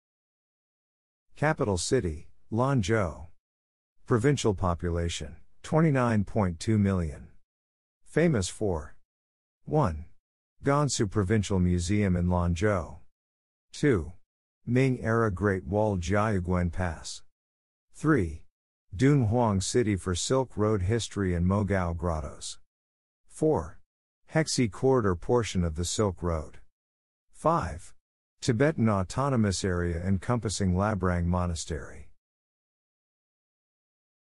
Gansu Province
Capital city: Lanzhou.
Provincial population: 29.2 million.
Famous for:.
1. Gansu Provincial Museum in Lanzhou.
2. Ming-era Great Wall Jiayuguan Pass.
3. Dunhuang City for Silk Road history and Mogao Grottoes.
4. Hexi Corridor portion of the Silk Road.
5. Tibetan Autonomous Area encompassing Labrang Monastery.